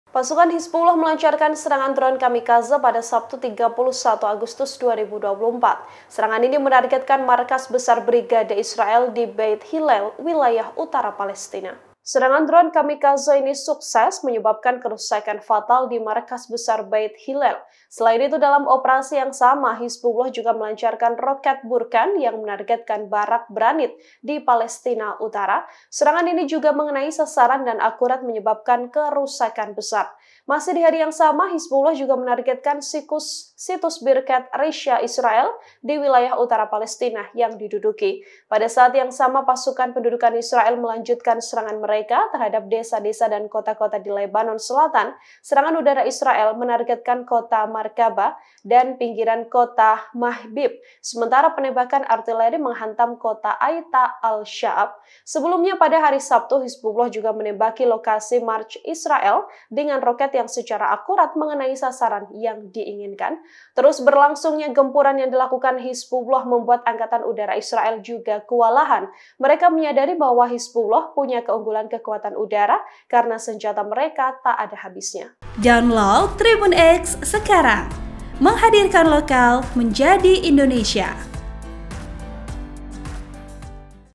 Iron Dome Jebol Angkatan Udara Nyerah, Markas Besar Israel di Beit Hilel Diserbu Kamikaze Hizbullah
TRIBUN-VIDEO.COM - Pasukan Hizbullah melancarkan serangan drone Kamikaze pada Sabtu (31/8/2024).

Serangan ini menargetkan markas besar Brigade Israel di Beit Hilel, wilayah utara Palestina.

Serangan drone Kamikaze ini sukses menyebabkan kerusakan fatal di Markas Besar Beit Hilel.

Selain itu, dalam operasi yang sama, Hizbullah juga melancarkan roket Burkan yang menargetkan Barak Branit di Palestina Utara.

Serangan ini juga mengenai sasaran dan akurat menyebabkan kerusakan besar.

Masih di hari yang sama, Hizbullah juga menargetkan situs Birket Risha Israel di wilayah utara Palestina yang diduduki.

Pada saat yang sama, pasukan pendudukan Israel melanjutkan serangan mereka terhadap desa-desa dan kota-kota di Lebanon selatan.

Serangan udara Israel menargetkan kota Markaba dan pinggiran kota Mhaybib, sementara penembakan artileri menghantam kota Aita al-Shaab.

Sebelumnya pada hari Sabtu, Hizbullah juga menembaki lokasi Marj Israel dengan roket yang secara akurat mengenai sasaran yang diinginkan.

Terus berlangsungnya gempuran yang dilakukan Hizbullah membuat angkatan udara Israel juga kewalahan.

Mereka menyadari bahwa Hizbullah punya keunggulan kekuatan udara karena senjata mereka tak ada habisnya. (Tribun-Video.com)

Editor Video: Putri Anggun Absari